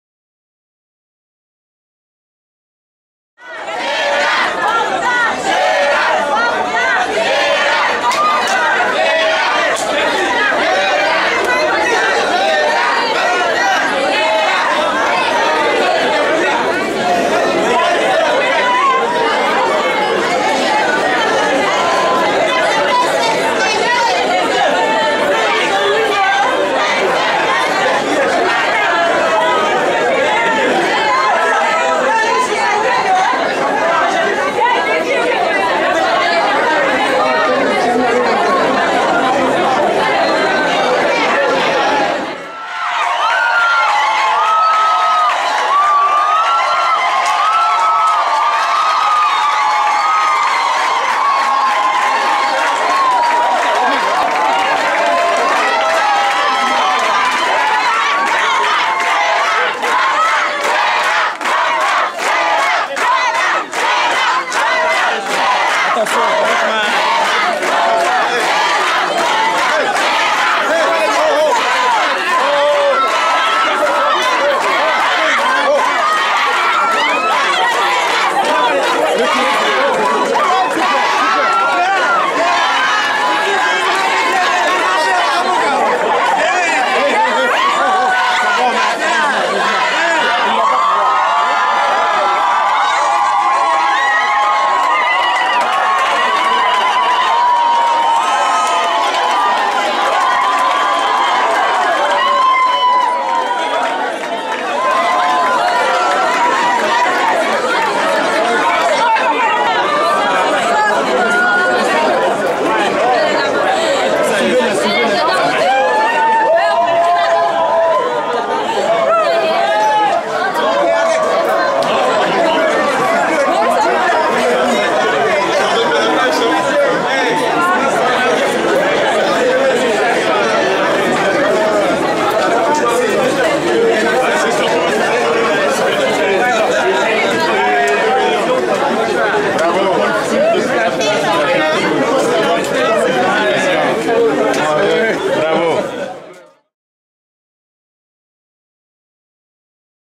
Accueil Triomphal de Faso Soap à l'aéroport de Ouagadougou.
Lundi 15 Avril 2013. Il est minuit quand Moctar et Gérard, étudiants 2iE vainqueurs de la GSVC 2013 à Berkeley (USA), descendent de l'avion à l'aéroport de Ouagadougou. Pour la suite, regardez plutôt...